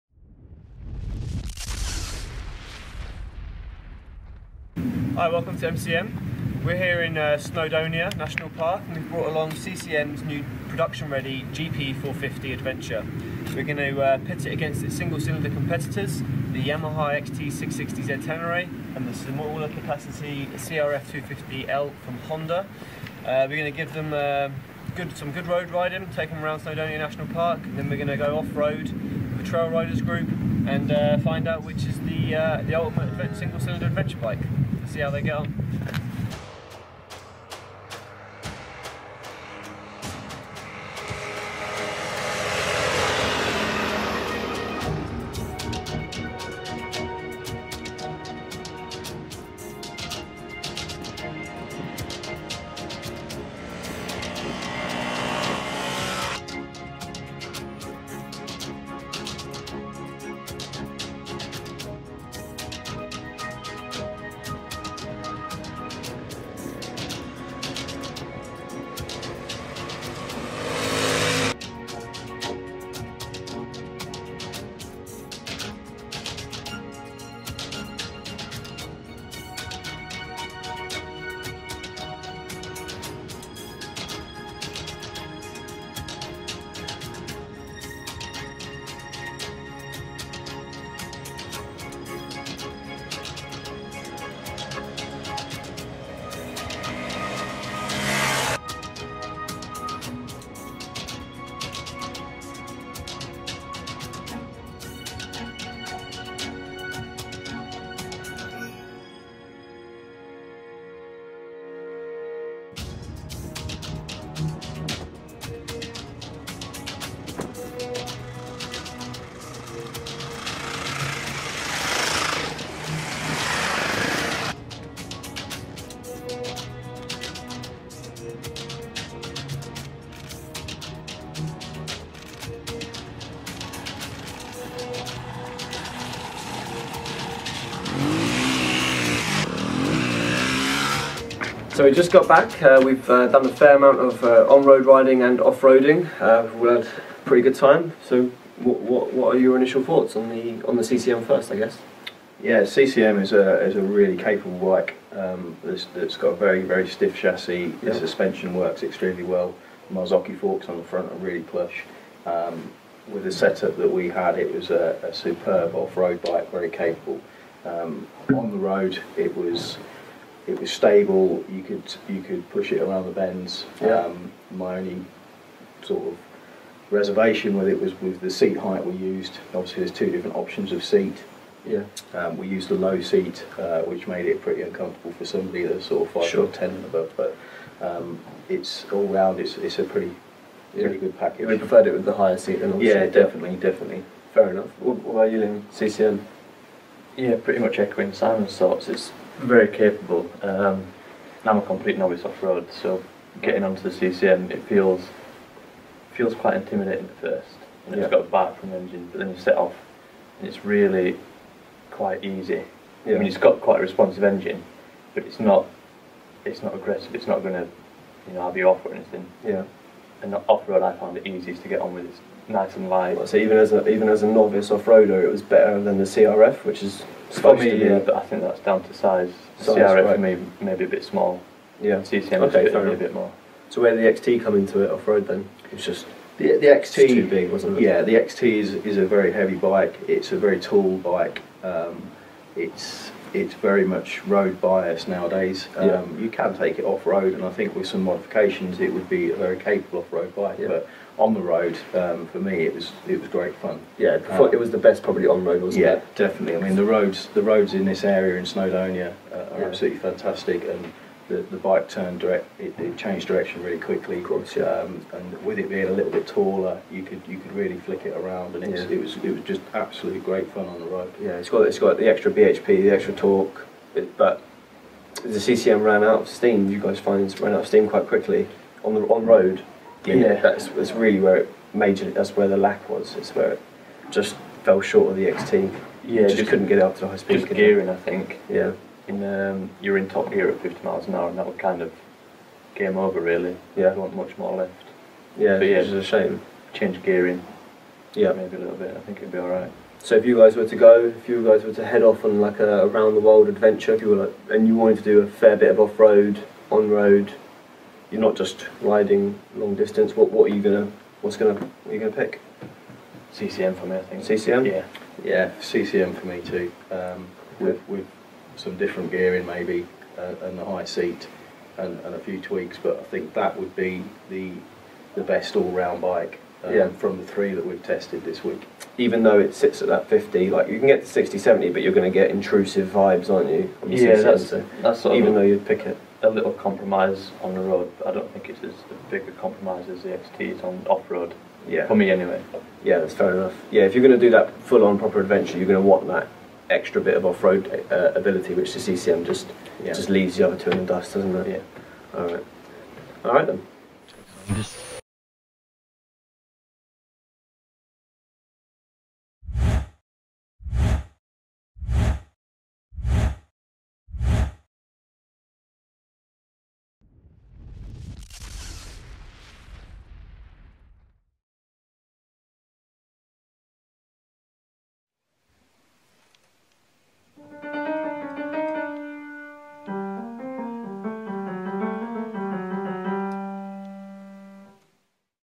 CCM GP450 Adventure takes on rivals | Interviews | Motorcyclenews.com
Britain's own CCM has joined the adventure bike market with this radical, new Dakar-inspired 450cc single.

MCN had a first taste of the prototype towards the end of last year and now the production bike is ready it's time to find out where it fits in.

So we pitted it against the bigger Yamaha XT660Z Tenere and smaller Honda CRF250L trailie in North Wales to find out.